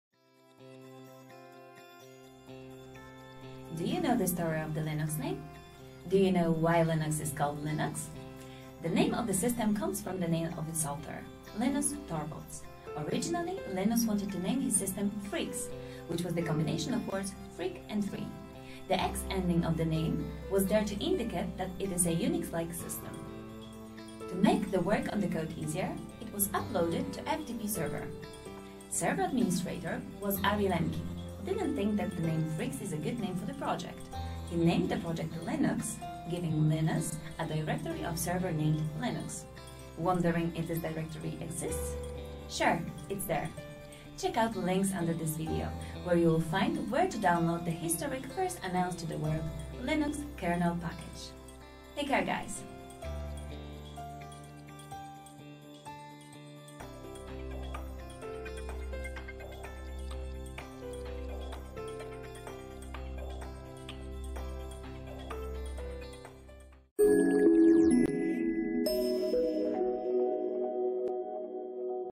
Short story of Linux name
Here are the links we have mentioned in the video:
- ftp://ftp.funet.fi/pub/linux/ - first Linux codes share.
- ftp://ftp.funet.fi/pub/linux/kernel/Historic/ - historic, first announced to the world Linux kernel package.

for great tutorials programming tutorials.